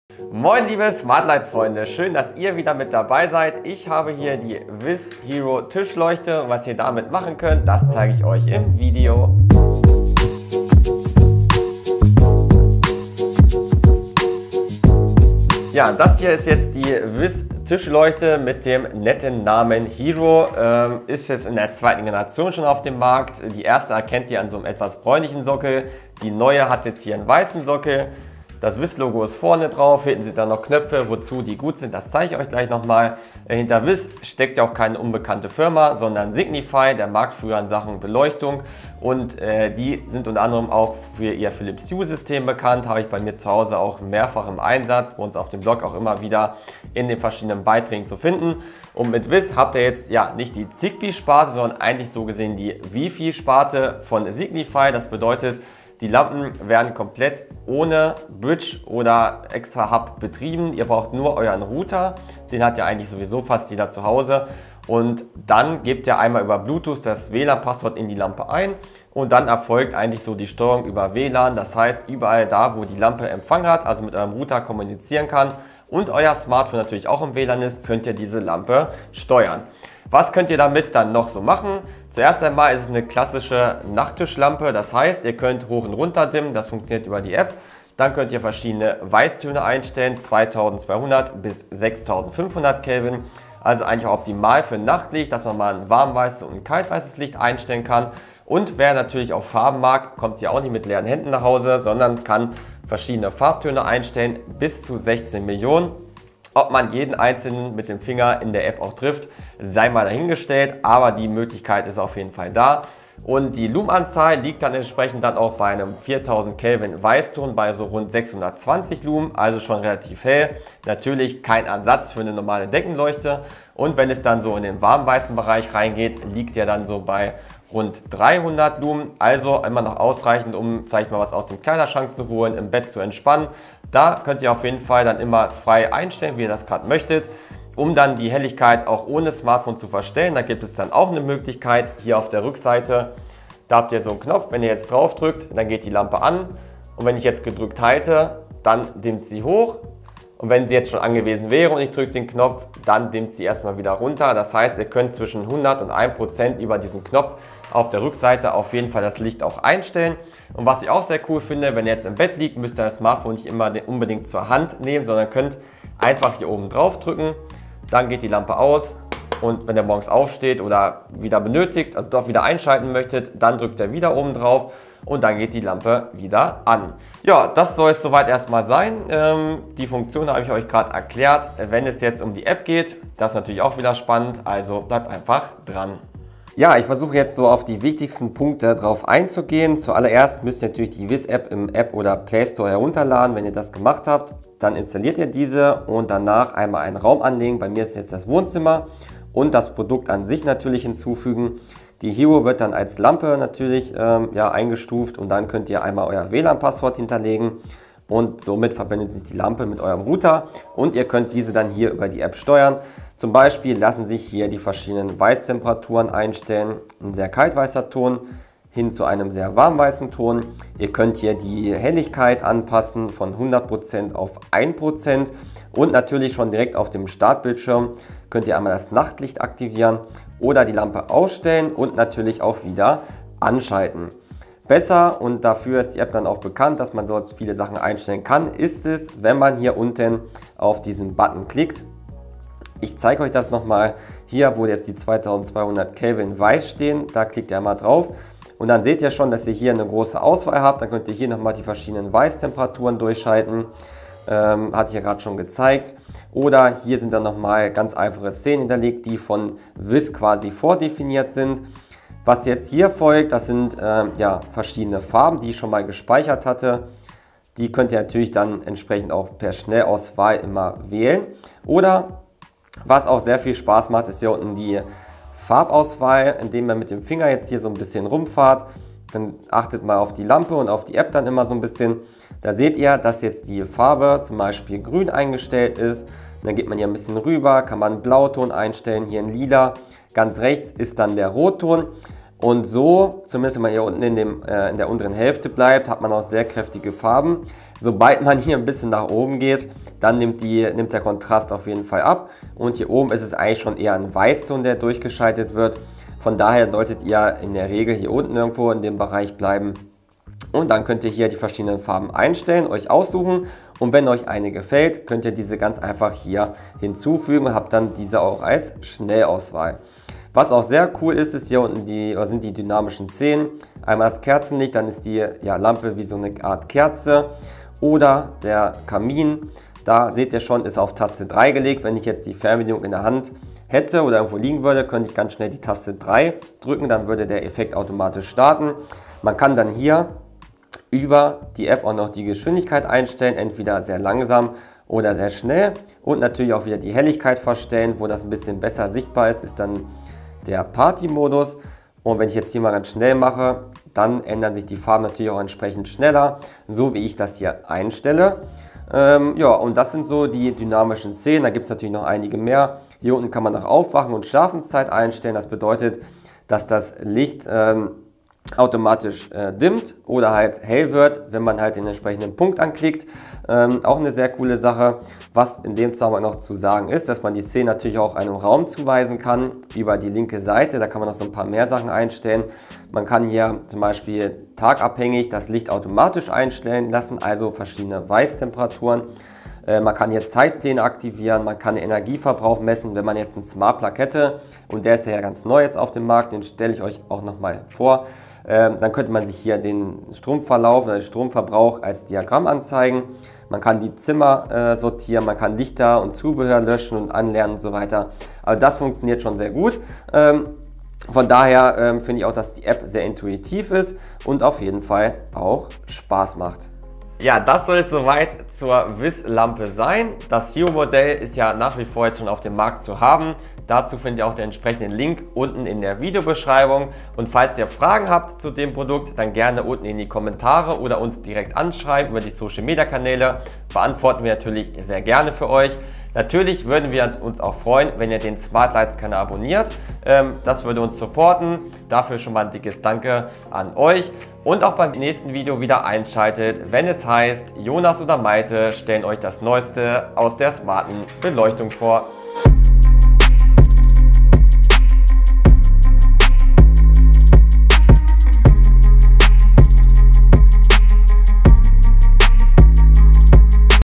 Ausgepackt: WiZ Hero Tischleuchte im Test
Mit der WiZ Hero Lampe bekommt ihr ein schickes Design und viele Funktionen nach Hause. Sie wird per WiFi angebunden und kann verschiedene Weißtöne und viele Farben wiedergeben. Im Video zeige ich euch diese und gehe auf die technischen Details ein. Mit der Philips Hue Go kann sie aber nicht verglichen werden, da sie keinen Akku eingebaut hat. Viel Spaß!

WiZ Hero Tischleuchte ⇒
Komplettes WiZ Sortiment ⇒
Komplettes Hue Sortiment ⇒

Timeline:
00:00 | 01. Intro
00:15 | 02. Technische Details
03:05 | 03. WiZ App & Farben
06:56 | 04. Outro